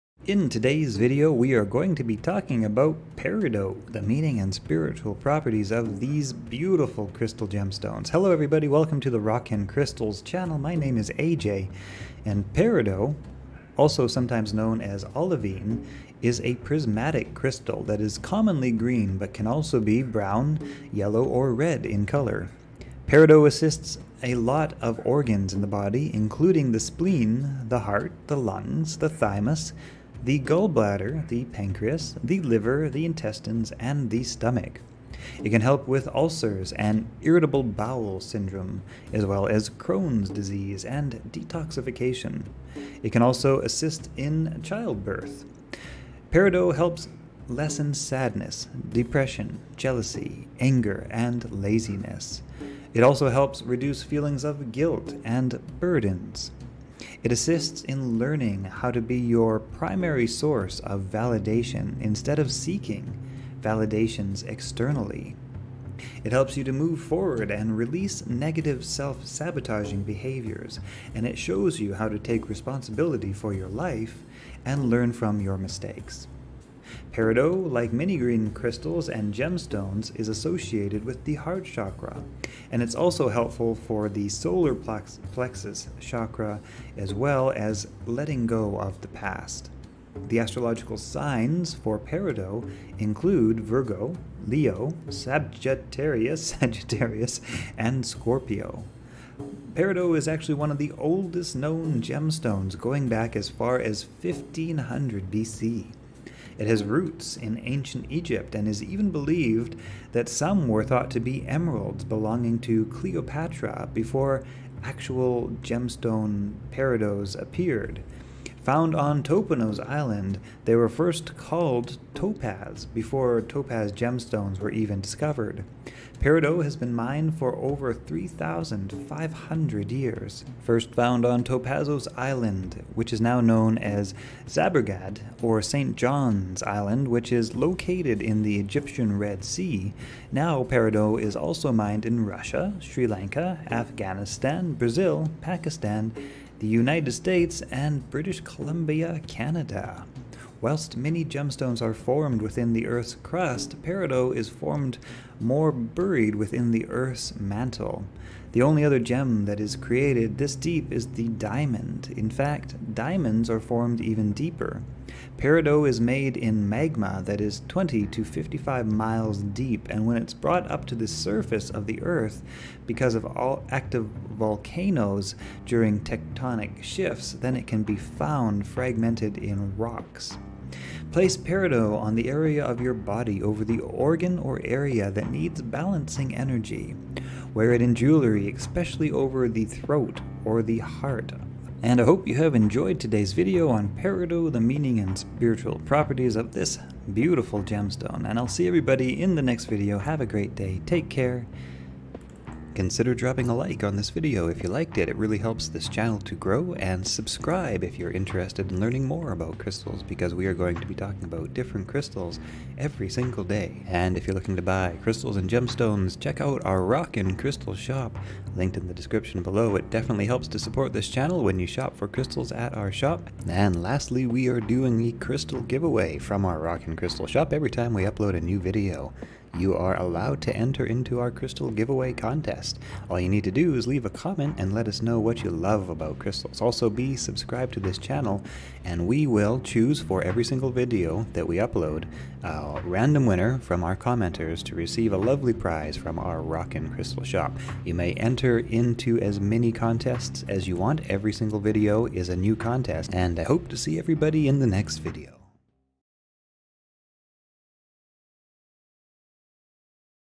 Peridot Meaning Benefits and Spiritual Properties
Chapters:

Peridot Meaning and Spiritual Properties

Also known as chrysolite or olivine, Peridot is a prismatic crystal that is most commonly green but can also be brown, yellow or red.

Physical Energy Properties: Peridot assists a lot of organs in the body including the spleen, heart, lungs, thymus, gallbladder, pancreas, liver, intestines, and the stomach. It can help with ulcers and irritable bowel syndrome as well as Crohn's disease and detoxification. Can also assist in childbirth and detoxing.

Emotional Energy Properties: Helps lessen sadness, depression, jealousy, anger, and laziness. Also helps reduce feelings of guilt and burdens. Assists in learning how to be your primary source of validation instead of seeking validation externally. Helps move forward and releases negative self-sabotaging behaviours. Shows you how to take responsibility for your life and learn from your mistakes.

Chakras: Like many green crystals and gemstones Peridot is associated with the Heart chakra. It’s also helpful to the Solar Plexus chakra and letting go of the past.

Astrological signs: Connected with Virgo, Leo, Sagittarius and Scorpio.

History: Peridot is actually one of the oldest known gemstones going back as far as 1500 BC. It has roots in Ancient Egypt, and it’s even believed by some that what were thought to be emeralds belonging to Cleopatra were actually peridot gemstones. Found on Topazos Island, it was also first called Topaz before Topaz gemstones were even discovered. Peridot has been mined there for over 3,500 years.

Where it's found: First found on Topazos Island which is now known as Zabargad or St. John's Island, which is located in the Egyptian Red Sea. Now peridot is mined in Russia, Sri Lanka, Afghanistan, Brazil, Pakistan and the United States.

Variations, Rarity and Value: While many gemstones are formed within the Earth’s crust, peridot is formed more buried within the Earth in the mantle. The only other gem that is created this deep is the Diamond, in fact, Diamonds are formed even deeper. Peridot is made in magma that is 20 to 55 miles deep and then it’s brought up to the surface of the Earth because of active volcanoes or tectonic shifts.

How to use it: Place Peridot on the area of your body over the organ or area that needs balancing energy. Wear it in jewelry especially over the throat and the heart or on the fingers.